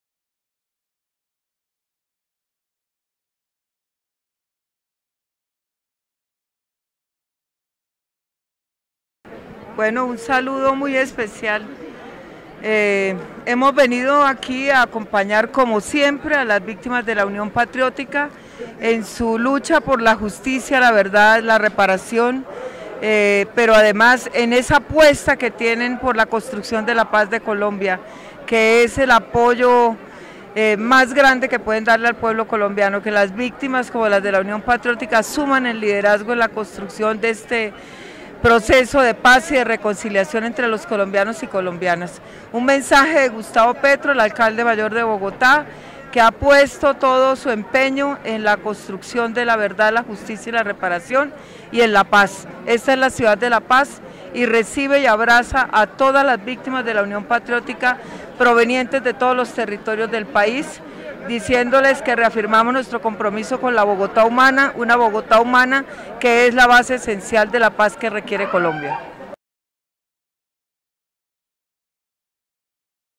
Gloria Flórez Schneider saludó las víctimas del Genocidio contra la Unión Patriótica.
En el marco del Día Nacional por la Dignidad de las Víctimas del Genocidio contra la Unión Patriótica la Secretaria Distrital de Gobierno, Gloria Flórez Schneider, acompañó a los afectados, en el acto público de reconocimiento y conmemoración realizado en la Plaza de Bolívar.